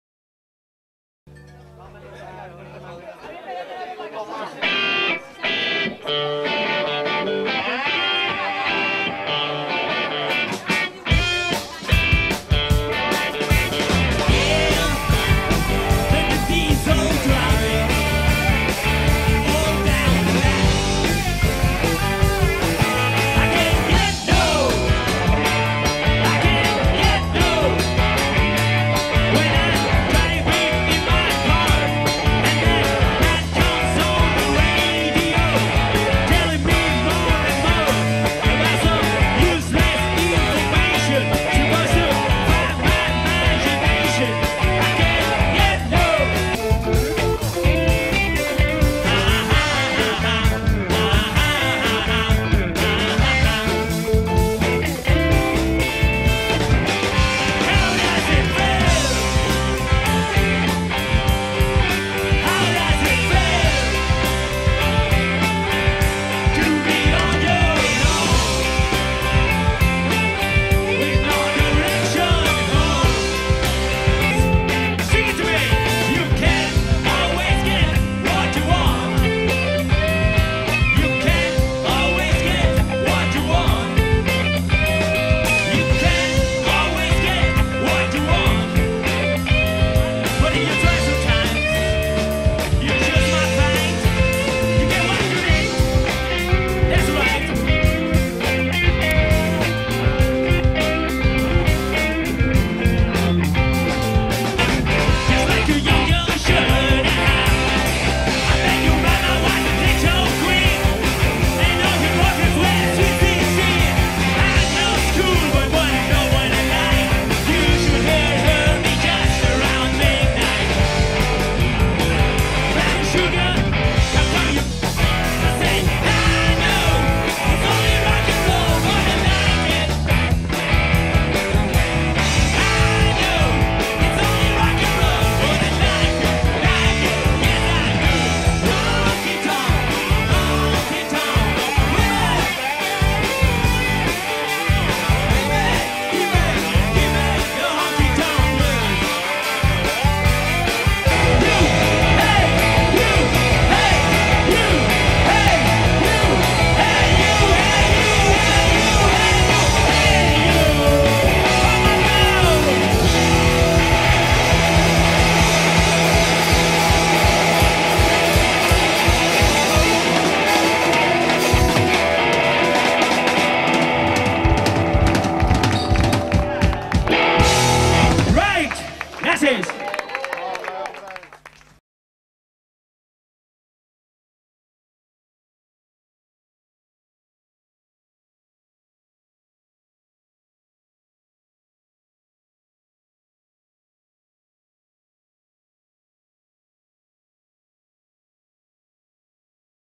Mickjägermeisters - Tributo a Rolling Stones
Xarxa Musical les presenta este fantástico tributo a la mítica banda The Rolling Stones